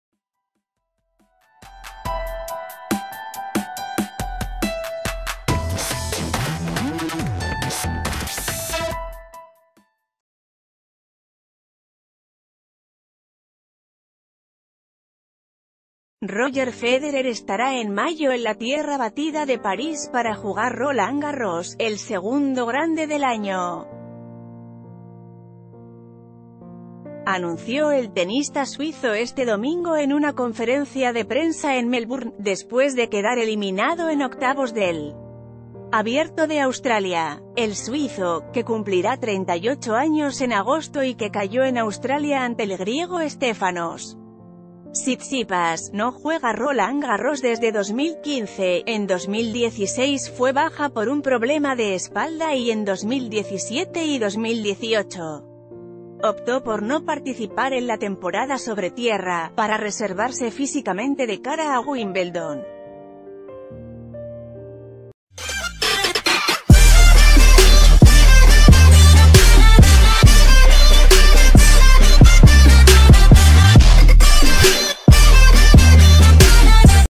Federer jugará Roland Garros este año
Roger Federer estará en mayo en la tierra batida de París para jugar Roland Garros, el segundo grande del año, anunció el tenista suizo este domingo en una conferencia de prensa en Melbourne, después de quedar eliminado en octavos del Abierto de Australia. El suizo, que cumplirá 38 años en agosto y ...